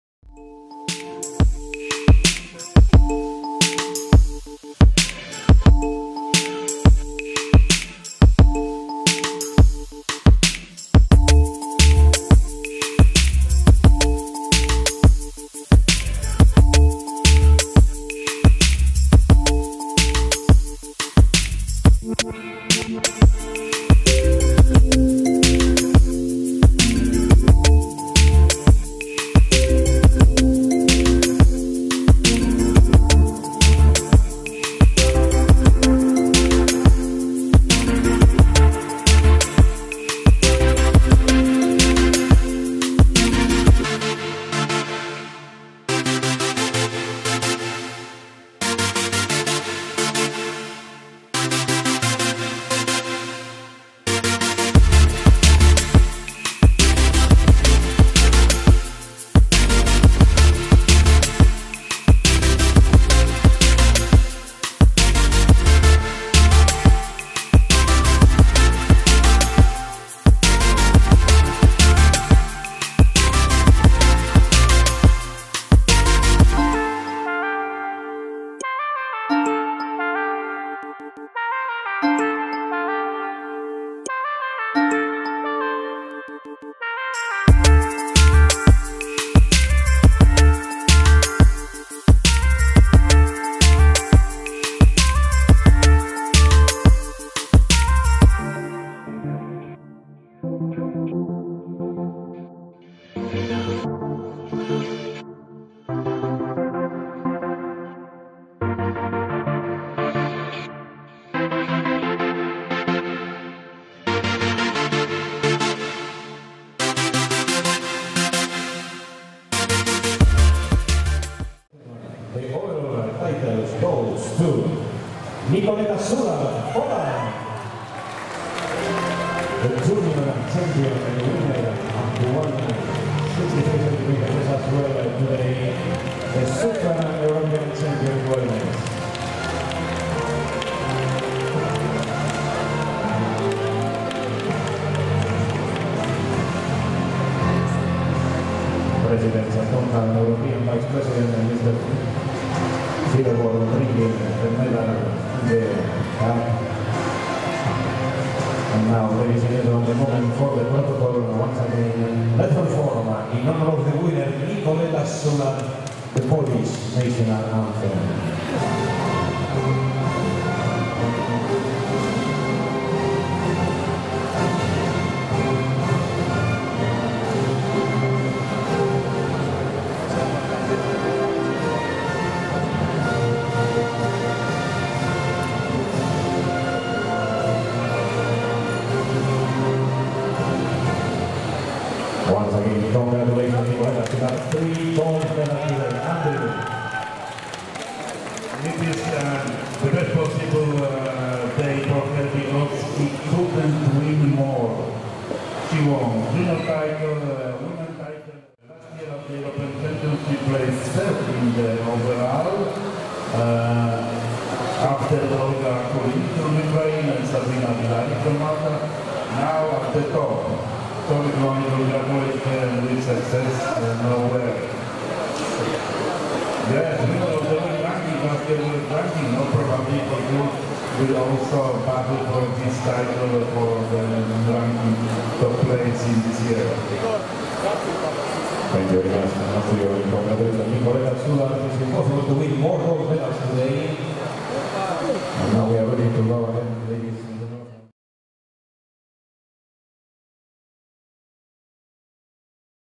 IFBB European Championships 2019 Women's Wellness Overall
📌 IFBB European Championships 2019
👉 Women's Wellness Overall Video
👉 🥇🏆 Nicoletta Sularz (Pomand)
👉 Competitor list
👉 Lara Santini (Croatia)
👉 Micaela Di Gennaro (Italy)
👉 Nikoletta Sularz (Poland )
👉 Dagmara Dominiczak (Poland)
📌 Elimination Rounds, Semifinals, Finals, Overalls and Awards Ceremonies
📌 BIKINI FITNESS, WELLNESS and BODYFITNESS